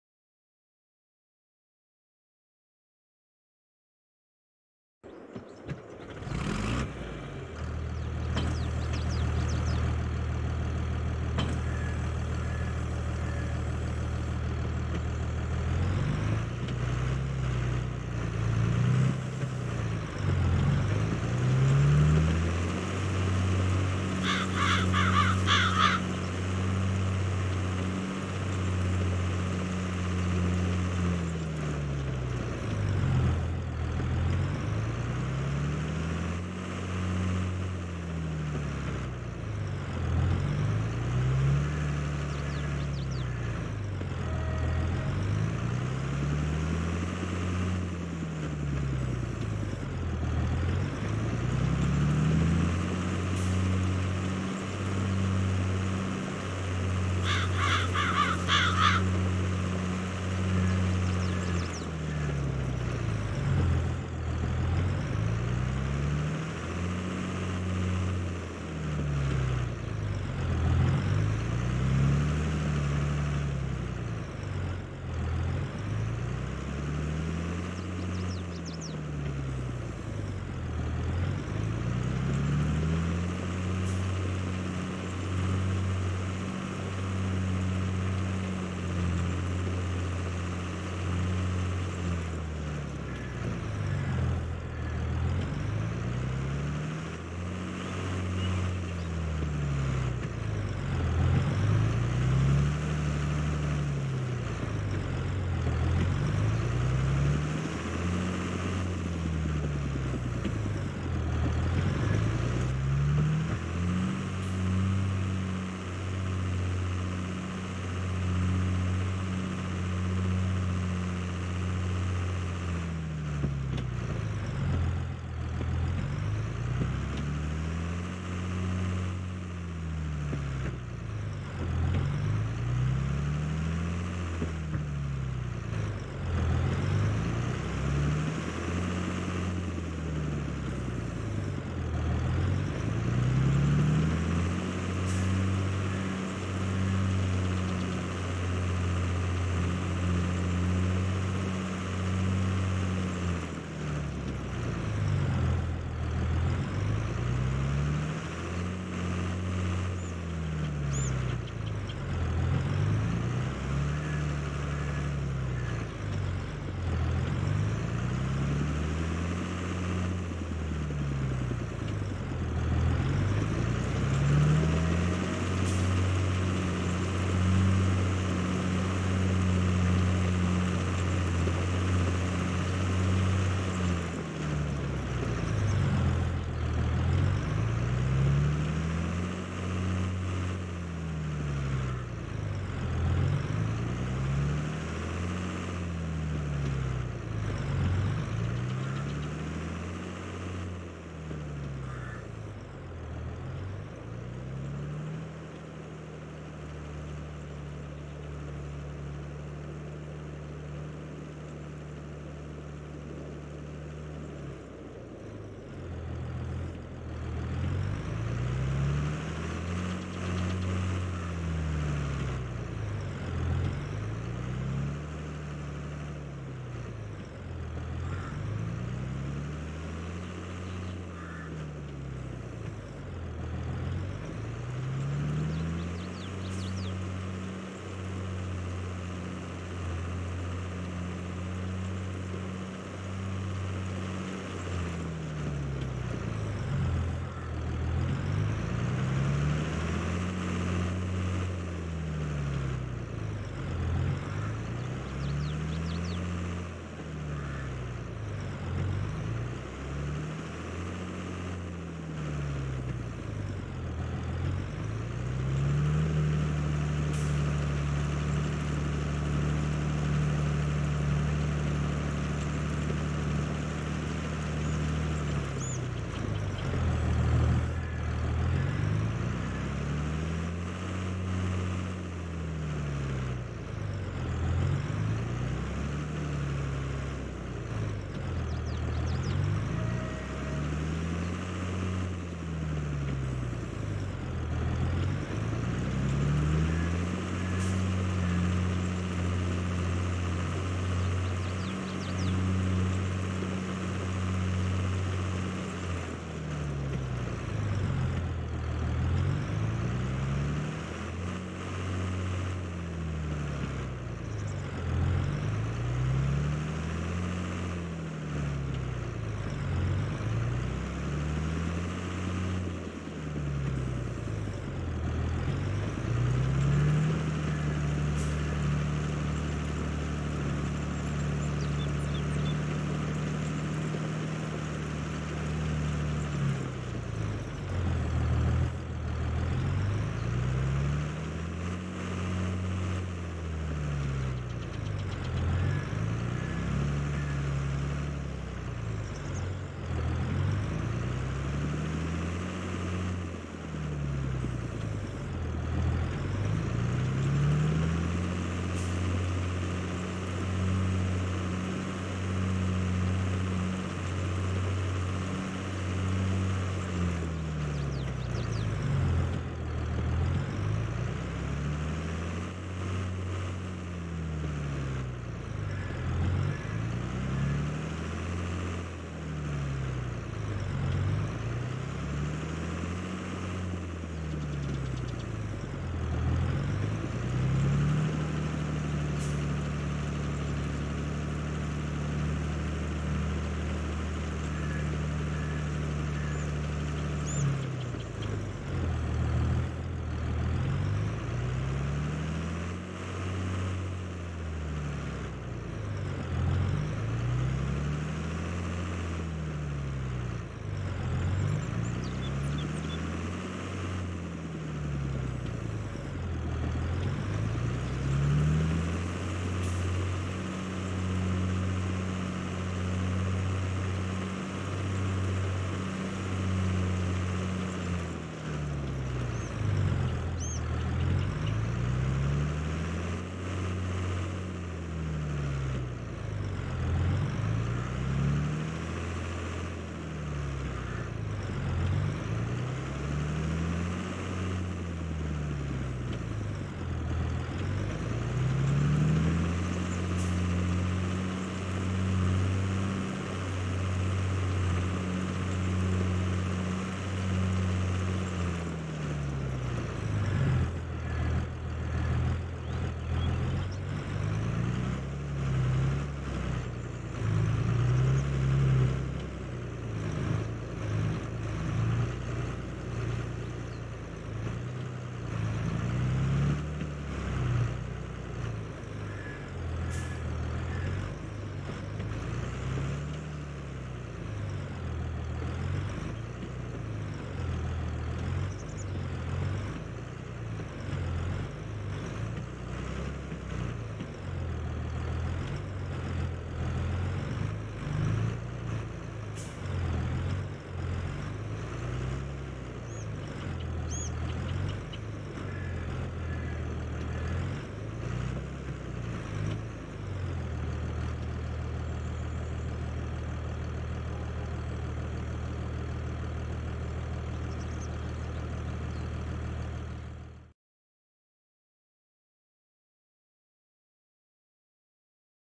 Tractor plowing field - Farming work - Harvester - Farm Machines - Kids Farm
Kids movie with a tractor working on a farm.
Farm machines
Tractor work
Farming